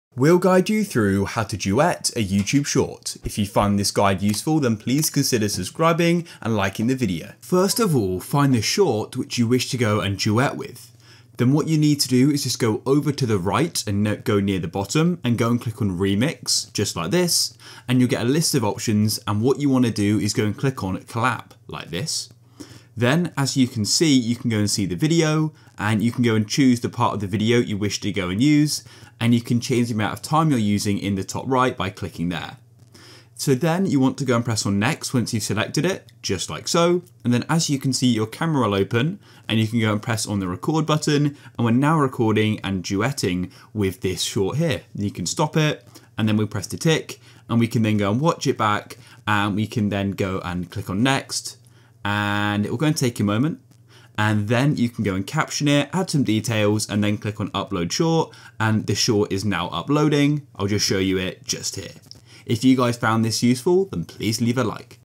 How To Duet Shorts In YouTube - Full Guide
Learn how to duet shorts in youtube in this video.

GuideRealm